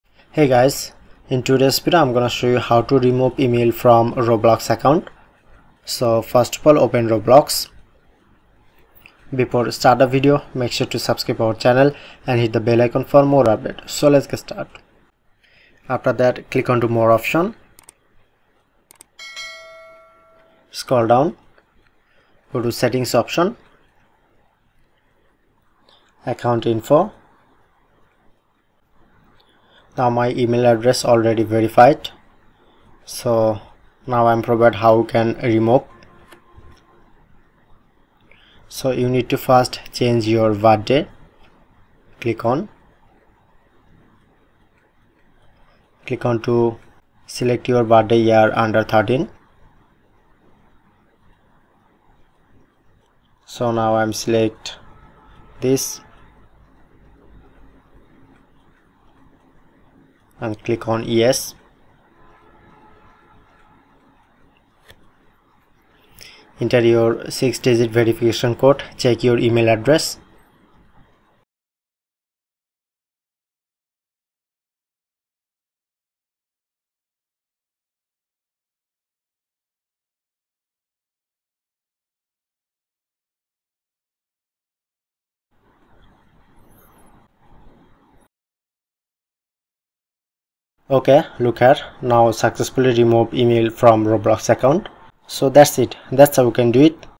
How to Remove Email from Roblox Account 2026 | Gull Guide
Video Parts:
0:00 Info & Intro
0:07 Open Roblox
0:27 Go To Account info
1:14 Check Email
1:34 Ending & Outro